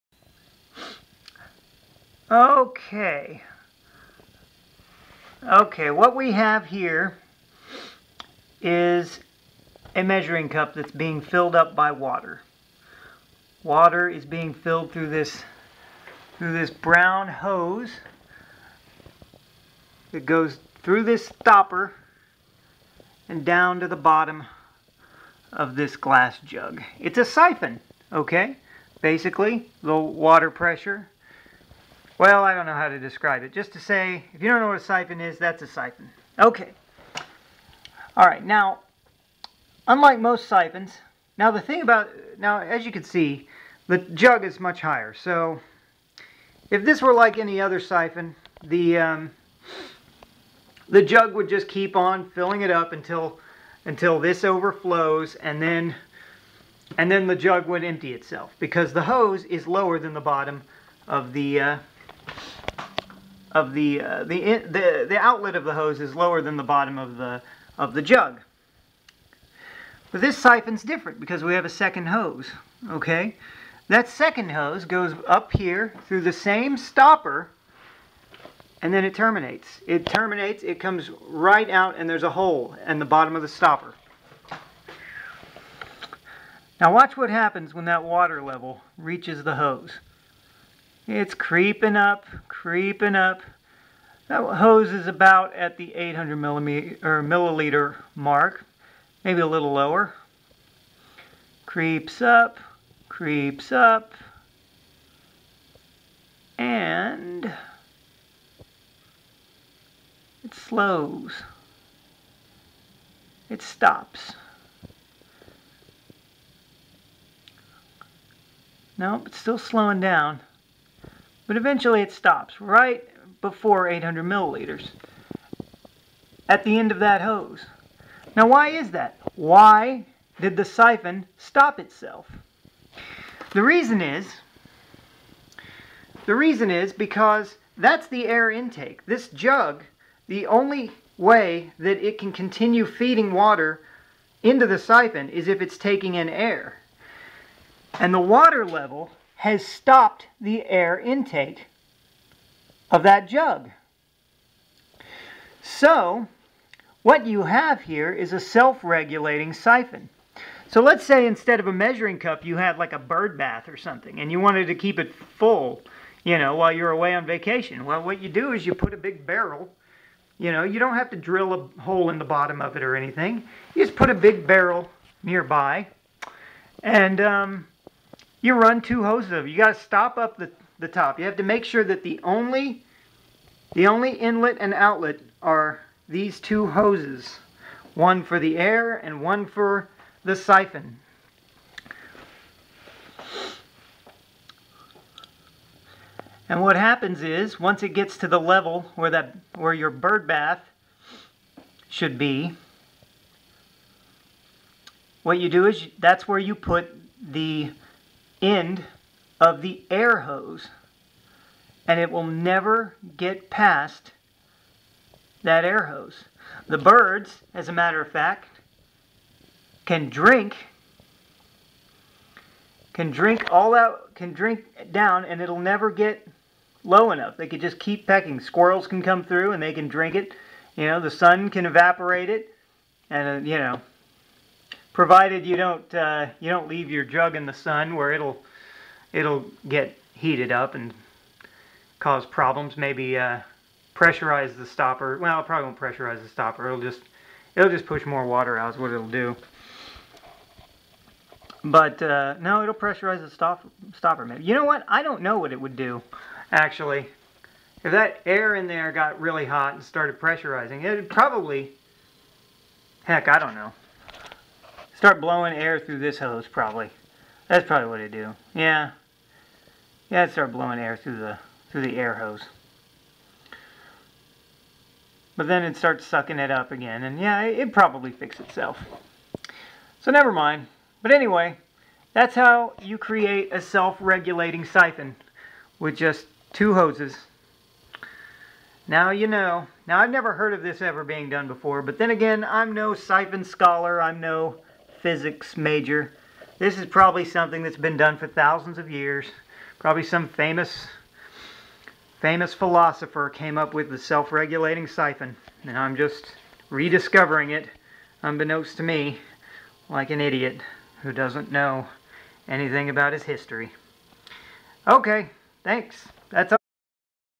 How a Self-Regulating Siphon Works
How to create a simple siphon that never fills the recieving vessel above a preset level.

Cheaper and less complicated than a float valve, and requires no water outlet at the bottom of the supply tank.

It also occurred to me later that you could do this with one hole at the top of the supply tank instead of two. The trick is that your air hose is wider (and shorter) than your siphon hose, and you feed your siphon hose through your air hose.